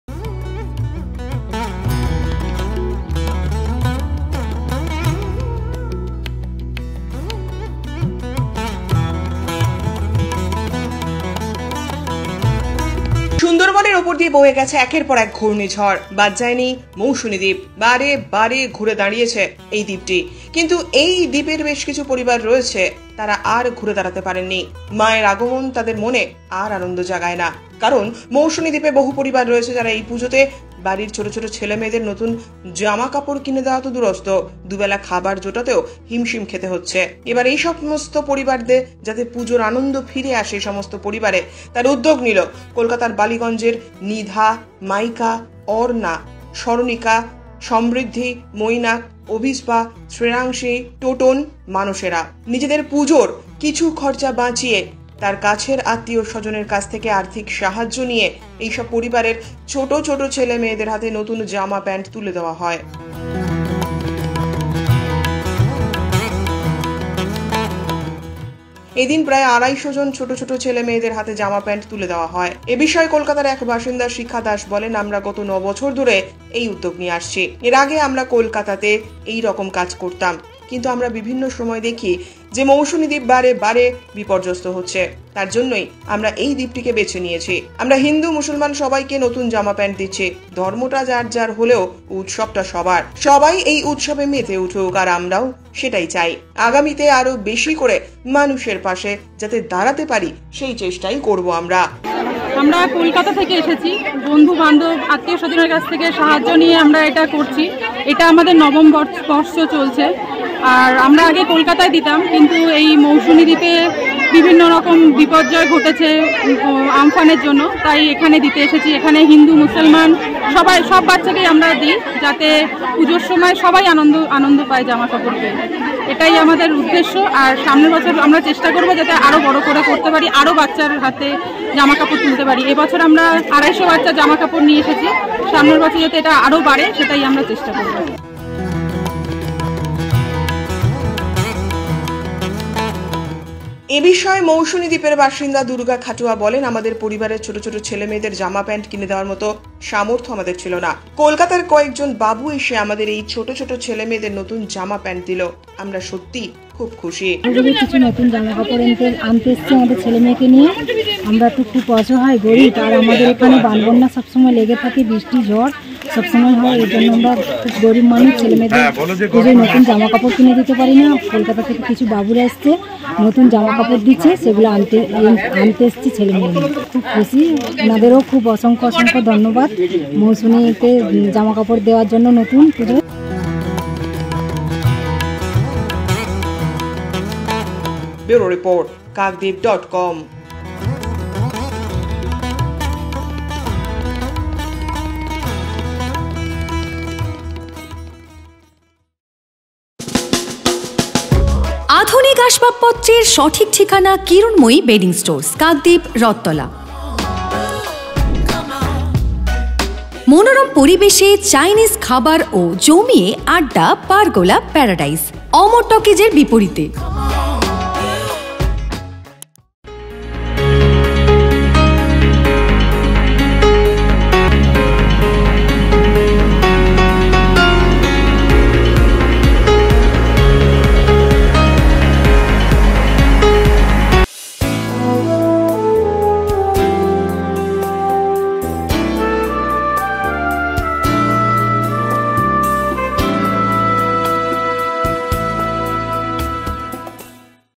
ছোটো ছোটো ছেলে মেয়েদের হাতে নতুন জামা প্যান্ট তুলে দিলেন কলকাতার বালিগঞ্জের কয়েকজন বাবু
ছোটো ছোটো ছেলে মেয়েদের হাতে নতুন জামা প্যান্ট তুলে দিলেন
কলকাতার বালিগঞ্জের কয়েকজন বাবু